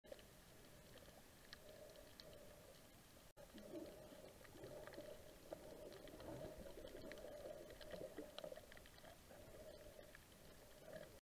Fourspot Butterflyfish Swimming in Hawaii
A pair of fourspot butterflyfish (Chaetodon quadrimaculatus) swimming over the coral reef at Kealakekua Bay (Captain Cook) near Kona, Hawaii.